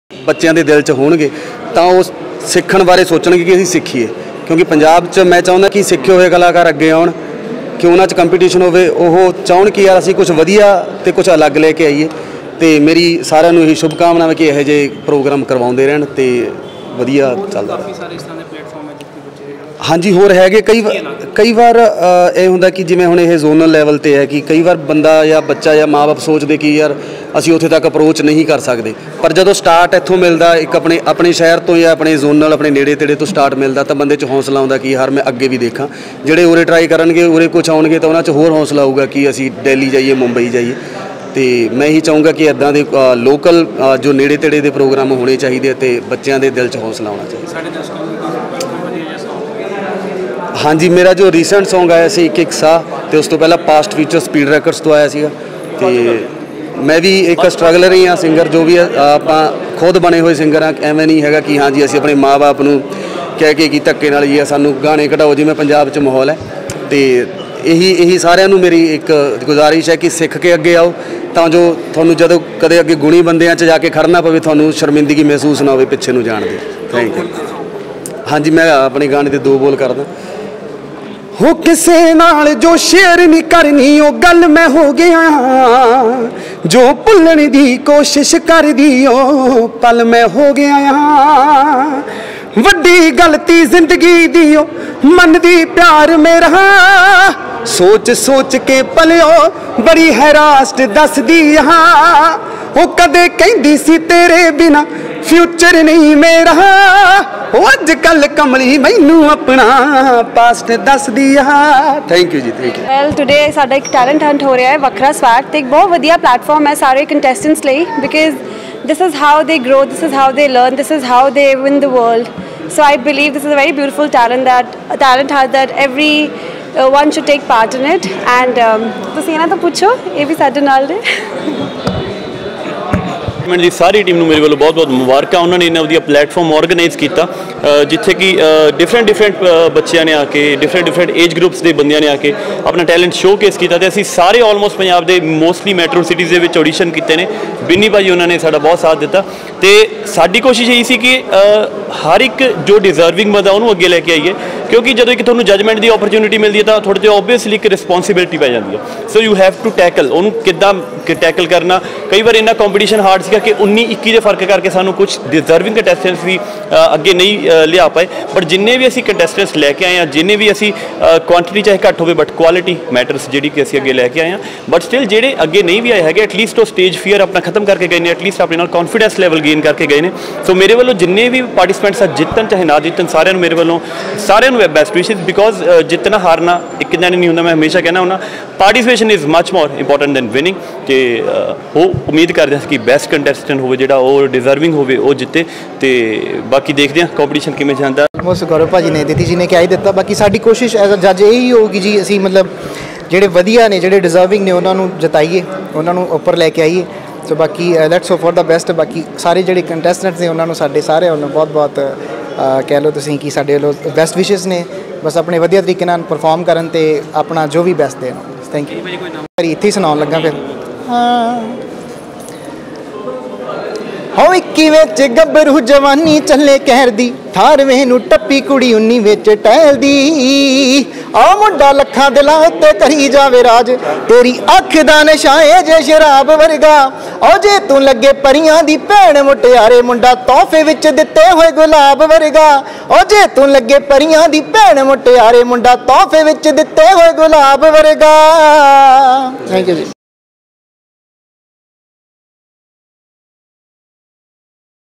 Wakhra Swag Talent Hunt | Miss India Aditi Sharma | Miel | Binnie Toor | Gaurav Nagpal
Wakhra Swag Talent hunt by Bulla entertainment. Here is an exclusive talk with the judges Miss India Chandigarh Aditi Sharma, Punjabi singer Miel, Singer Binnie Toor, Gaurav Nagpal, and Monty Sohal.